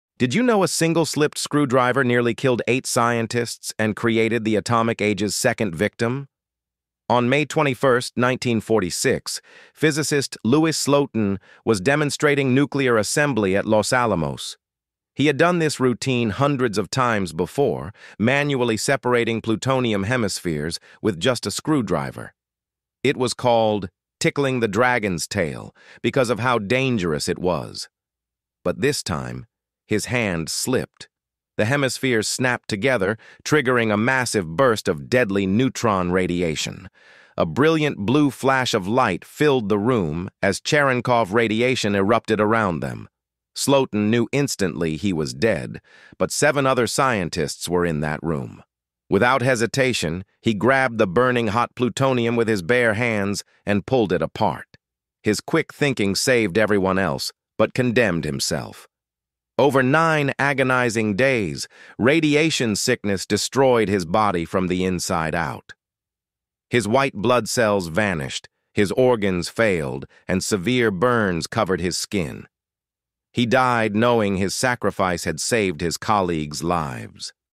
Scientist's Hand Slipped and Nearly Killed 8 People With Nuclear Radiation
one screwdriver slip almost ended everything